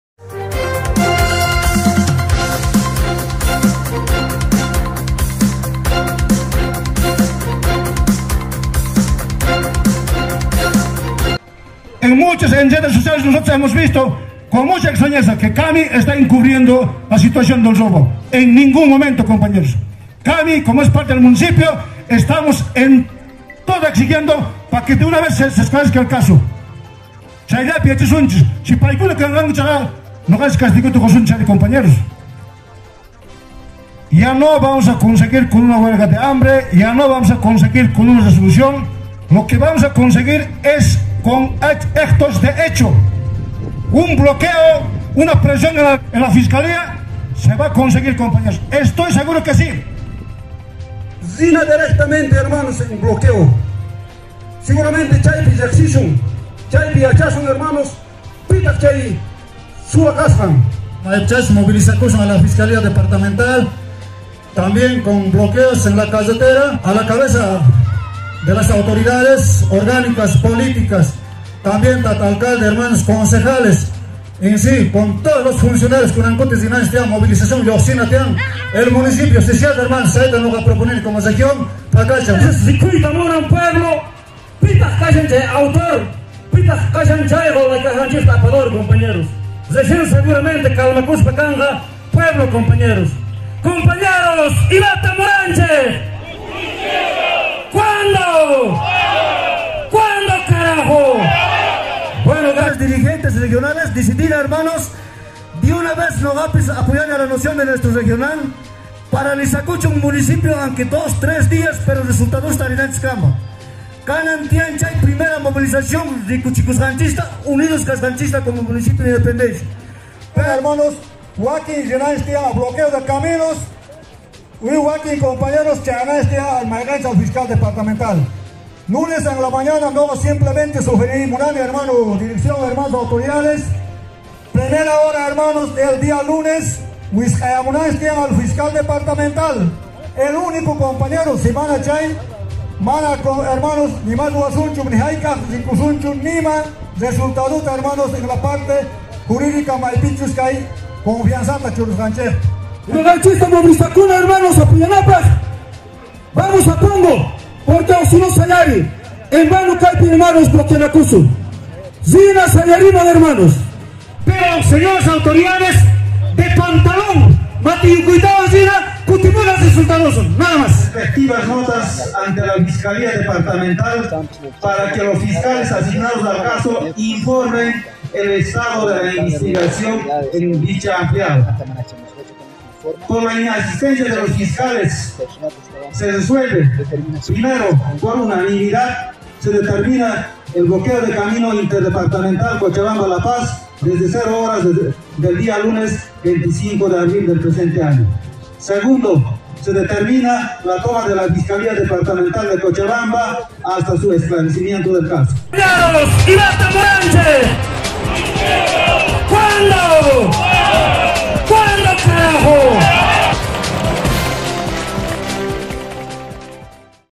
✓ INDEPENDENCIA| Anuncian bloqueo de caminos y toma de la fiscalía Departamental de Cochabamba🇮🇩
la Reunion de las  organizaciones sociales instituciones, comités cívicos en Independencia determinan Bloqueo de Caminos en Sayari desde las cero horas el día lunes 25 de Abril y toma de la fiscalía Departamental.